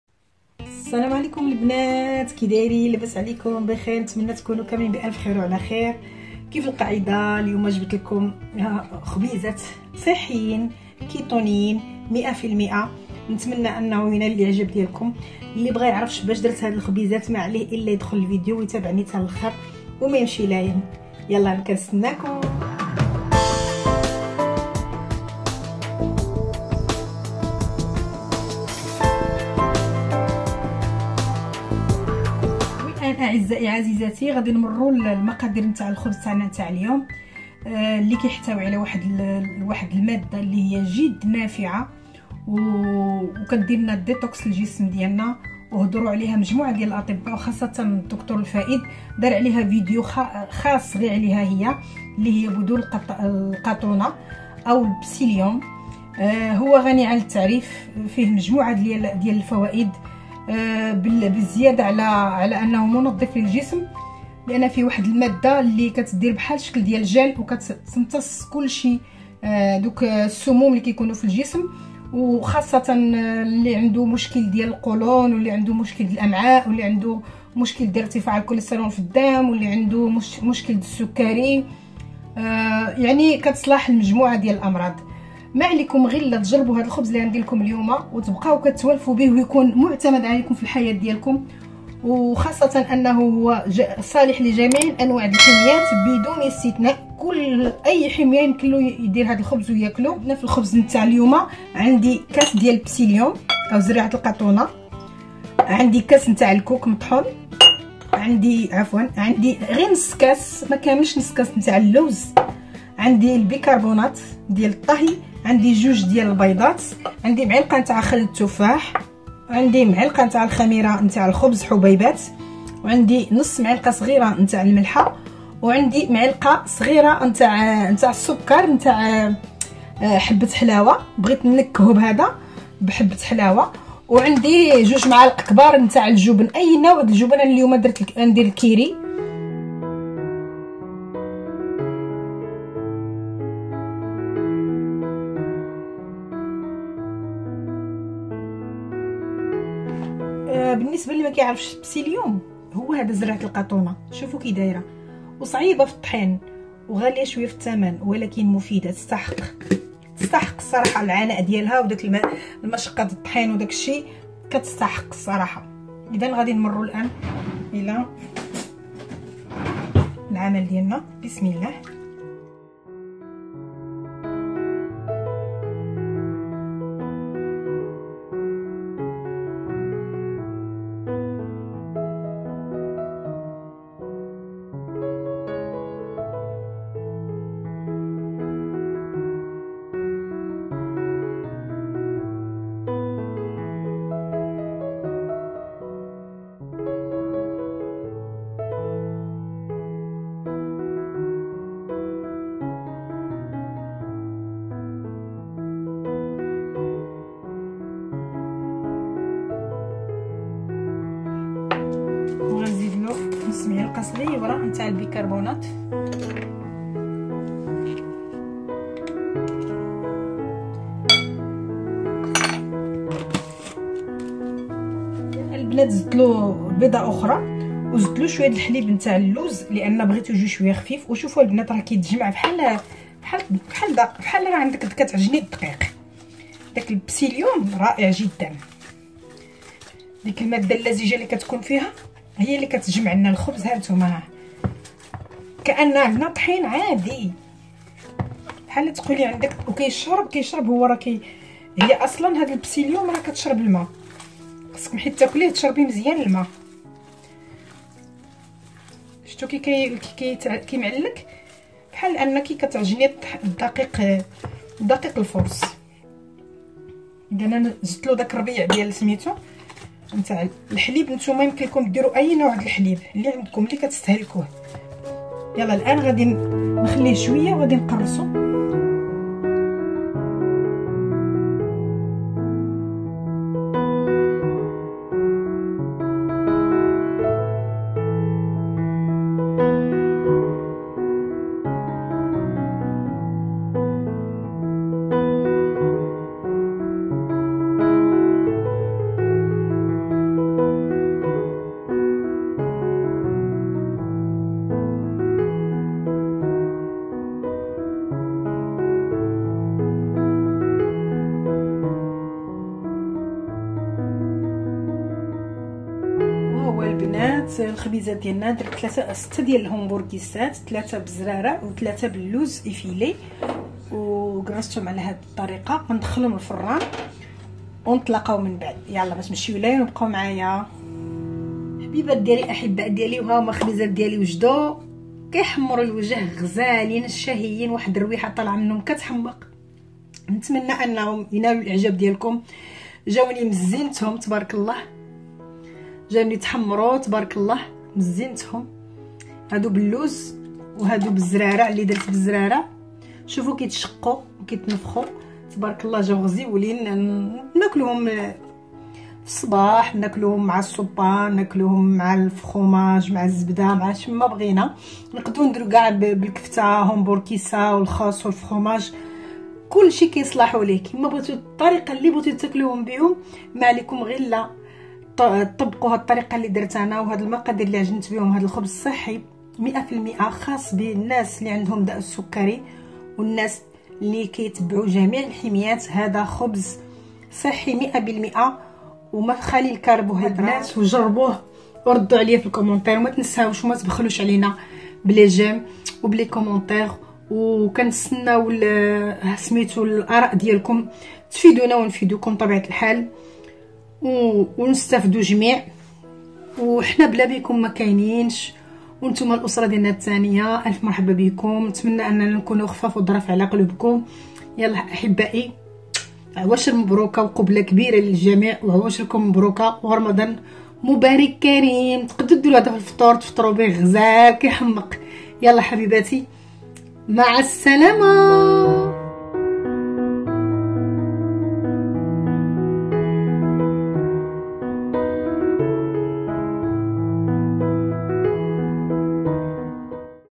خبز صحي لاصحاب الكيتو وكذا جميع الحميات بحبة القطونة او بسيليوم
المحتوى ؛ خبز صحي لاصحاب الكيتو وكذا جميع الحميات بحبة القطونة او بسيليوم  والكوك واللوز
 صالح كذلك لمرضى السكري المرجو مشاركة الفيديو مع الاصدقاء والاحباب ولا تبخلوا علينا بالضغط على زر المشاركة وكذا اشارة الاعجاب وشكرا .
المقادير : بيضتان / نصف كاس دقيق اللوز /كاس دقيق الكوك / كاس دقيق بذرة القطونة او بسيليوم / ملعقتان  كبيرة جبن كريمي /  ملعقة صغيرة خل التفاح / ملعقة صغيرة خميرة الخبز الجافة /  ملعقة صغيرة حبة حلاوة مدقوقة / قليل من الملح  او نصف ملعقة صغيرة / بيكاربونات غذائية قليل منها .